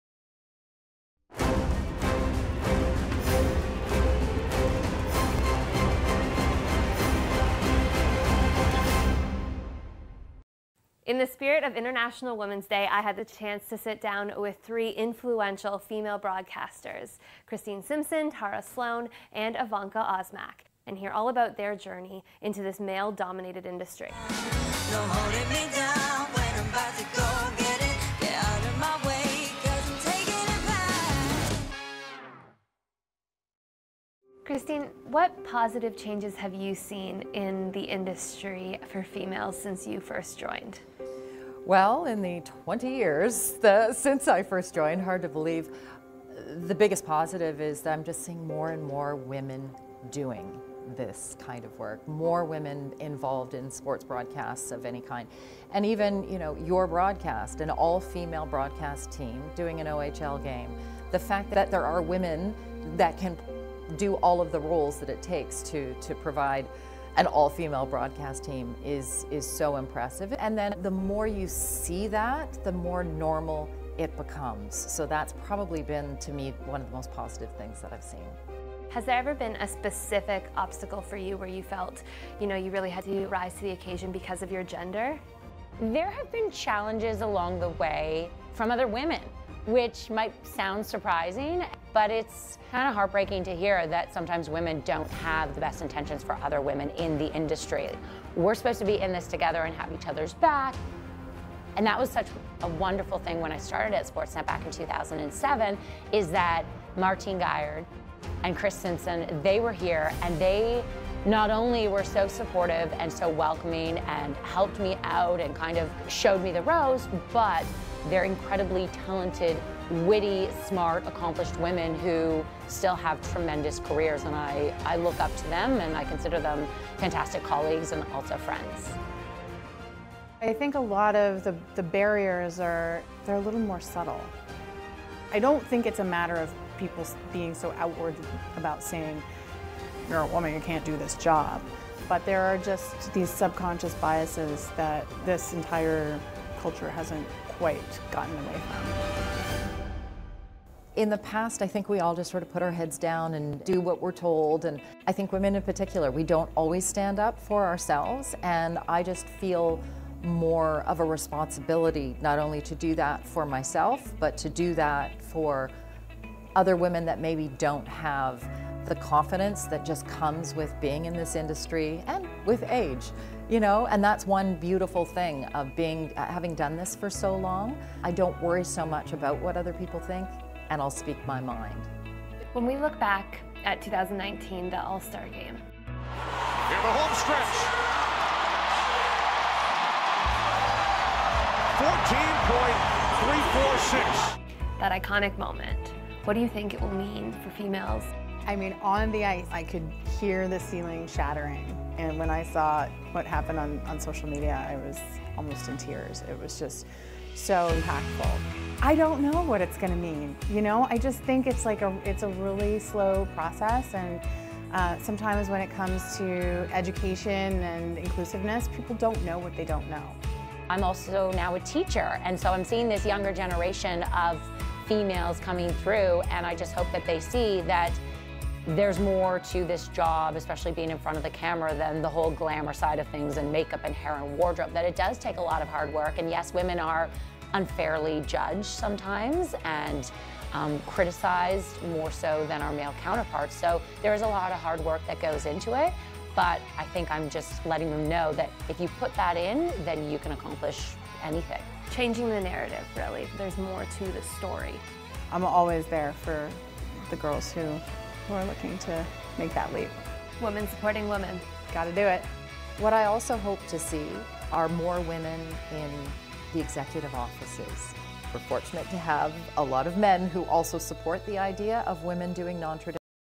The Female Influence In Sports Media
Aly Munro sits down on International Women's Day to speak with Chris Simpson, Tara Slone and Evanka Osmak, to discuss the challenges and triumphs they've experienced as females in sports media.